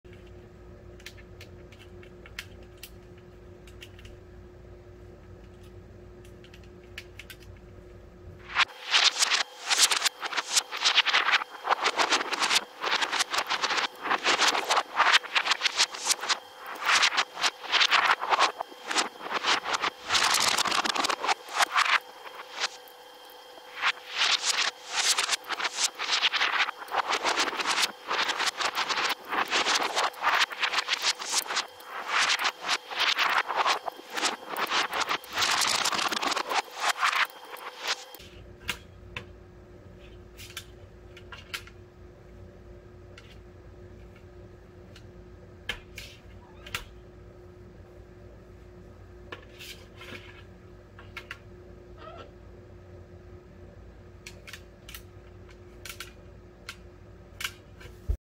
Satisfying Parrot Video
Cheeto the Umbrella Cockatoo Parrot empties a bucket of microfibers in record time! Entertaining Video.